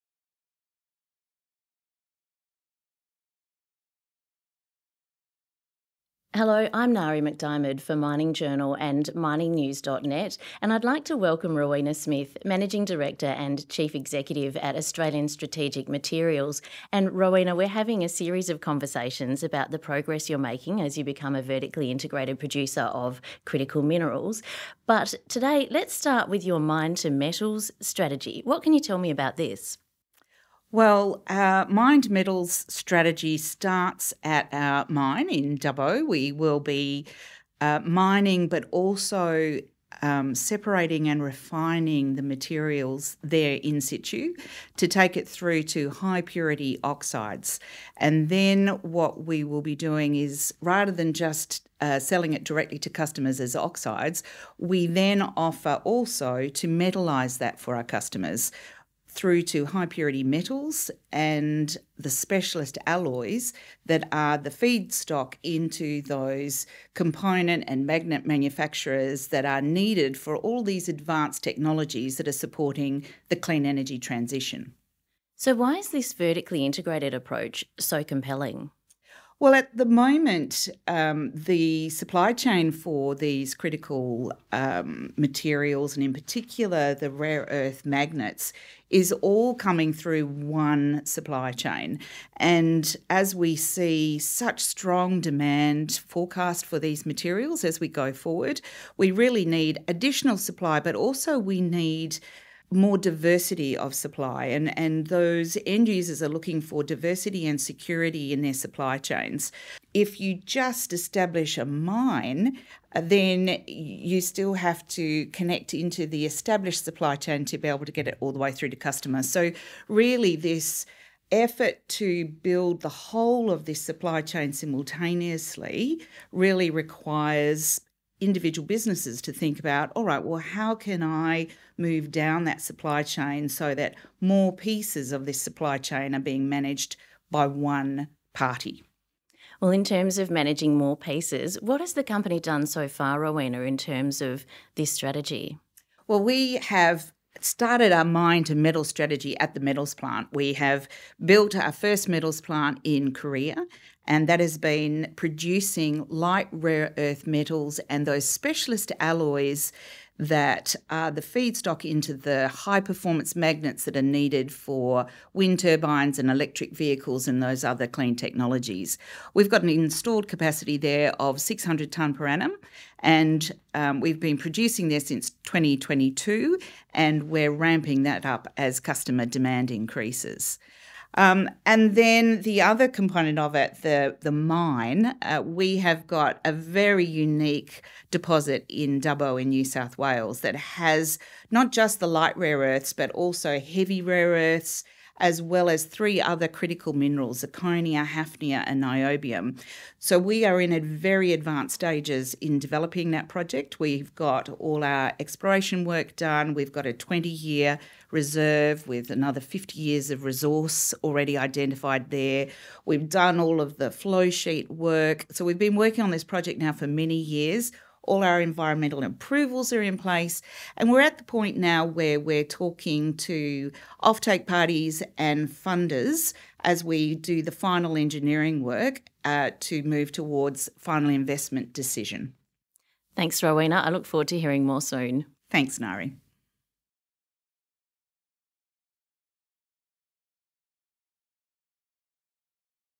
ASM delivering on mine to metals strategy
Emerging vertically integrated rare earths and critical minerals producer, Australian Strategic Materials, is already delivering on its “mine to metals” strategy, MD and CEO Rowena Smith explains.

ASM is advancing its Dubbo deposit and meanwhile delivered its first sale of neodymium praseodymium metal from its new Korean Metals Plant in September 2022.